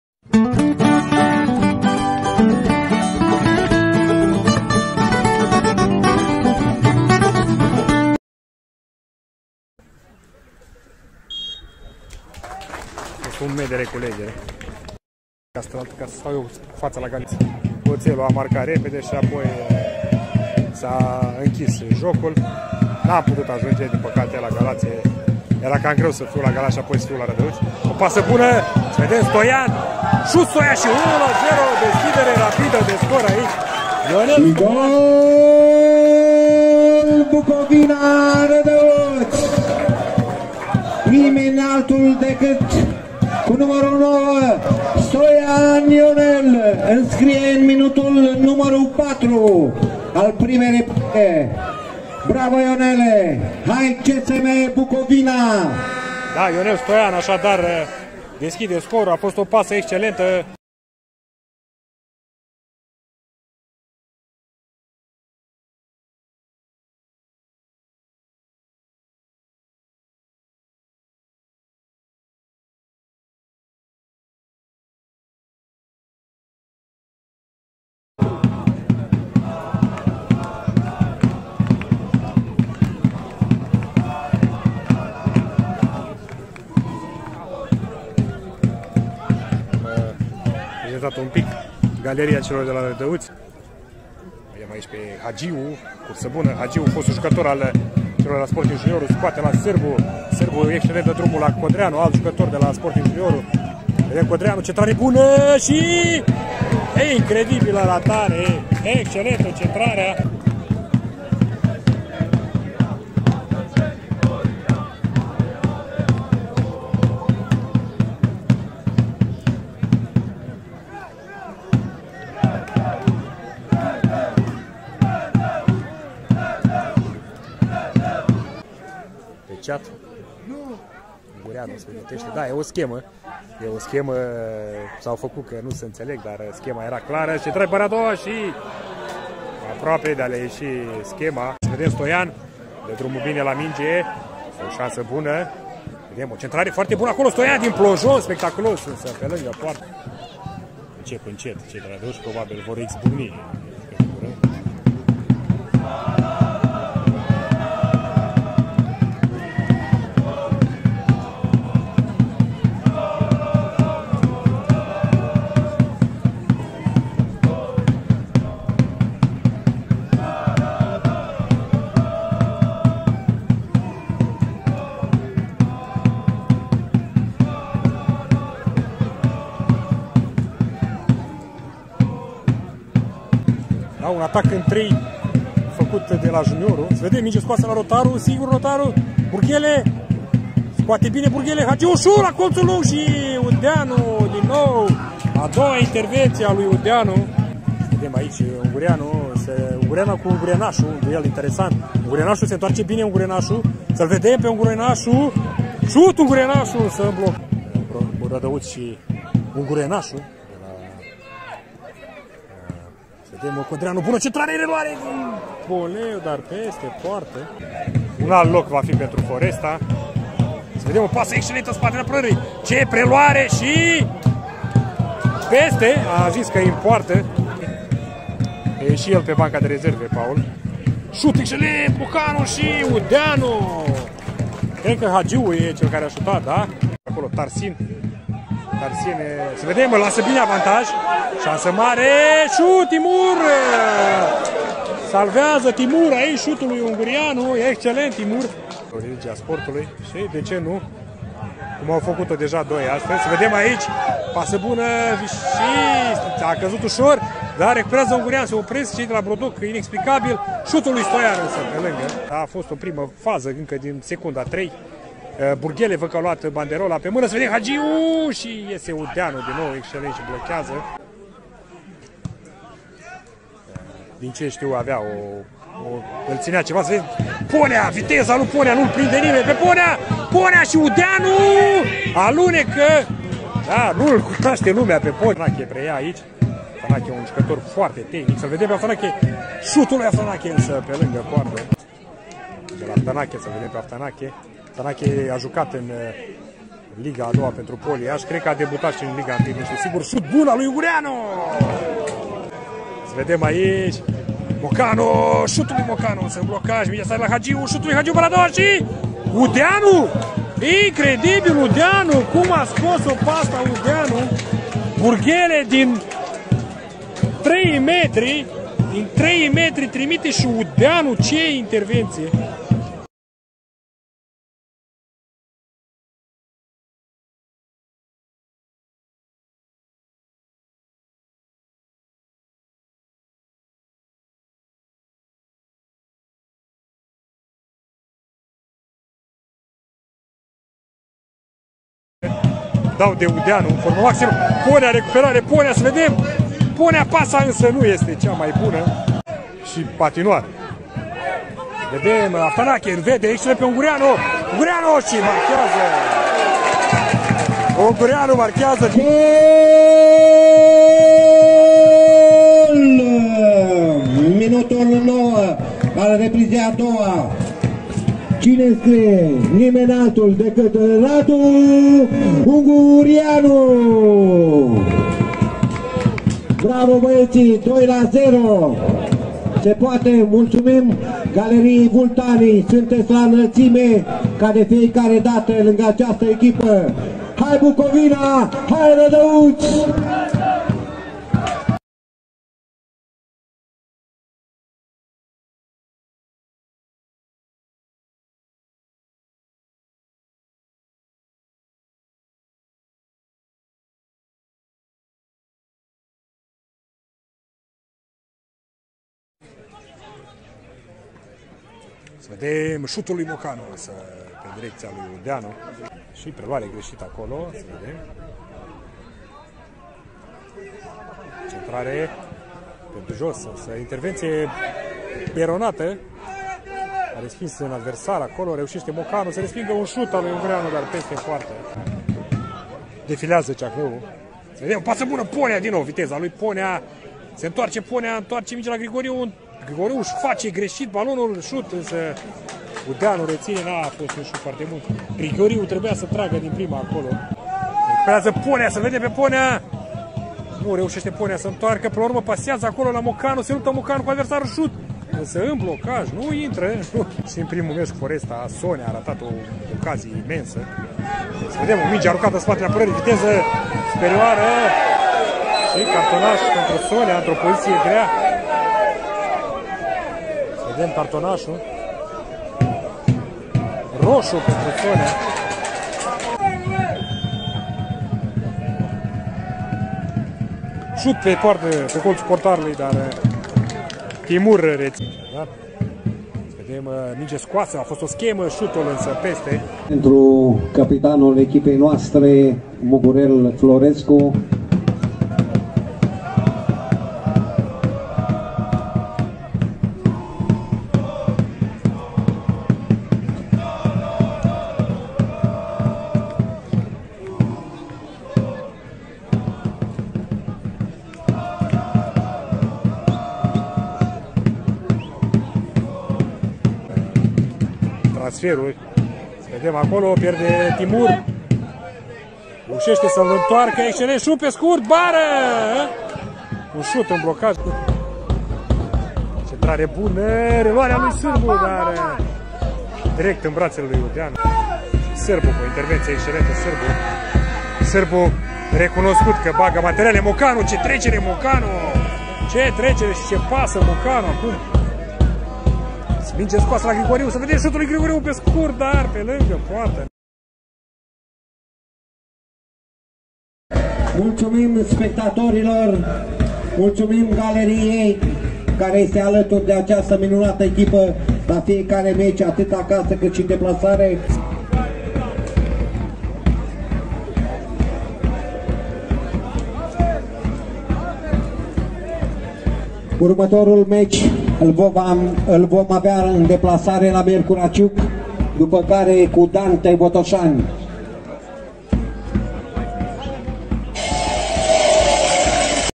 #553 Fotbal - Liga 3: Bucovina Radauti - Rapid Brodoc 2-0! Aftanache cu pasele, Udeanu cu paradele!
Intr-un meci din etapa a doua din Liga 3 - Seria 1, Bucovina Radauti s-a impus cu 2-0 in fata nou-promovatei Rapid Brodoc (jud. Vaslui)!